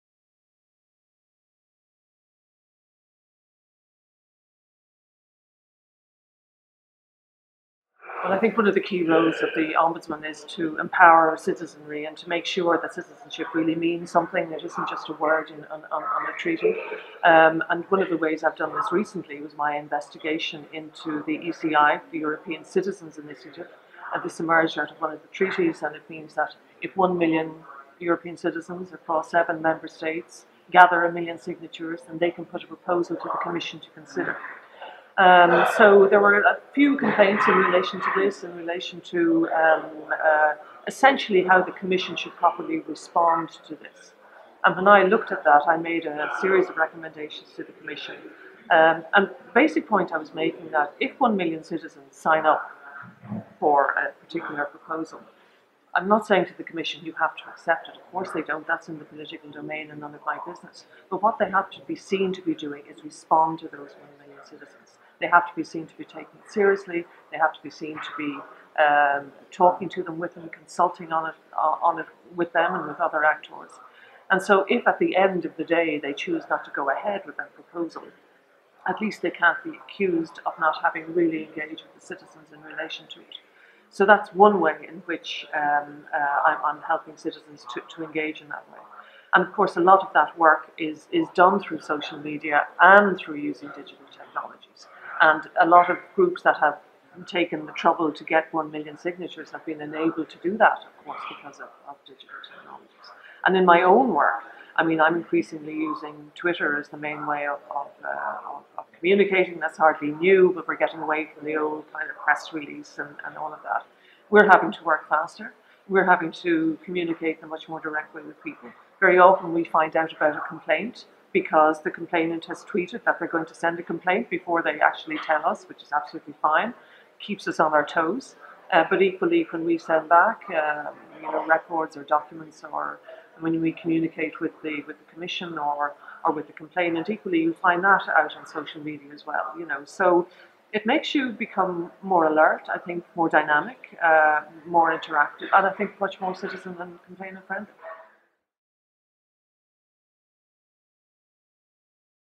A conversation with Emily O'Reilly - Part 2
The Digital Post interviewed Mrs Emily O'Reilly, European Ombudsman at Web Summit 2015 in Dublin.